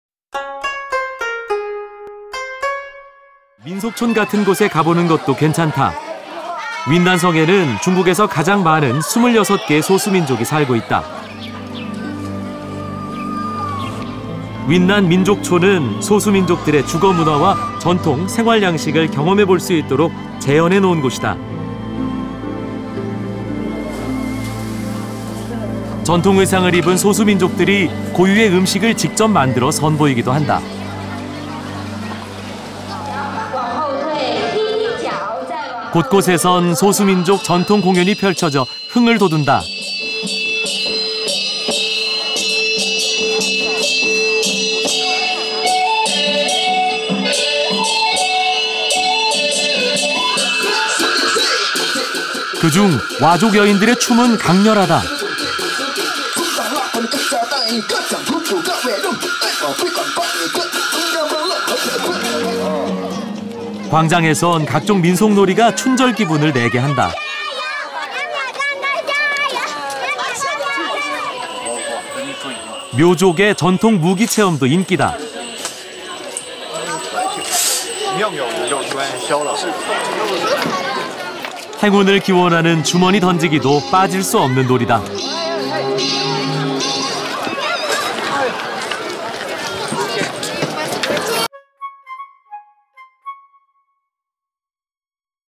【K】China Travel-Yunnan[중국 여행-윈난]소수민족의 삶, 윈난 민속촌/Yunnan/Folk Village/Yunnan Sheng/Ethnic Minorities
[한국어 정보]
민속촌 같은 곳에 가보는 것도 괜찮다. 윈난성에는 중국에서 가장 많은 26개의 소수민족이 살고 있다. 윈난 민속촌은 소수민족들의 주거문화와 전통, 생활양식을 경험해 볼 수 있도록
재현해 놓은 곳이다. 전통의상을 입은 소수민족들이 고유의 음식을 직접 만들어 선보이기도 한다. 곳곳에선 소수민족 전통공연이 펼쳐져 흥을 돋운다. 그 중 와족 여인들의 춤은 강렬하다. 광장에선 각종 민속놀이가 춘절 기분을 내게 한다. 묘족의 전통무기체험도 인기다. 행운을 기원하는 주머니 던지기도 빠질 수 없는 놀이다.

Such as folk go where anything is okay . Yunnan has the largest number of 26 ethnic groups live in China. Yunnan Folk Village is residential so you can see and experience the culture and traditional lifestyle of ethnic minorities
The place where it reproduces . Minorities in traditional costumes to create a unique showcase of the food directly in prayer . In a minority tradition spread throughout the show dotunda the excitement . The dance of the women of wa people is intense. In the square are various folk plays and the spring feel to me . The experience of Miao traditional weapons are popular. Good luck to bag toss game is also indispensable .
[Information]
■클립명: 아시아037-중국21-09 소수민족의 삶, 윈난 민속촌/Yunnan/Folk Village/Yunnan Sheng/Ethnic Minorities
■여행, 촬영, 편집, 원고: 손병규 PD (travel, filming, editing, writing: KBS TV Producer)

[Keywords]
아시아,Asia,동북아시아,중국,China,,,손병규,2015,3월 March,윈난성,Yunnan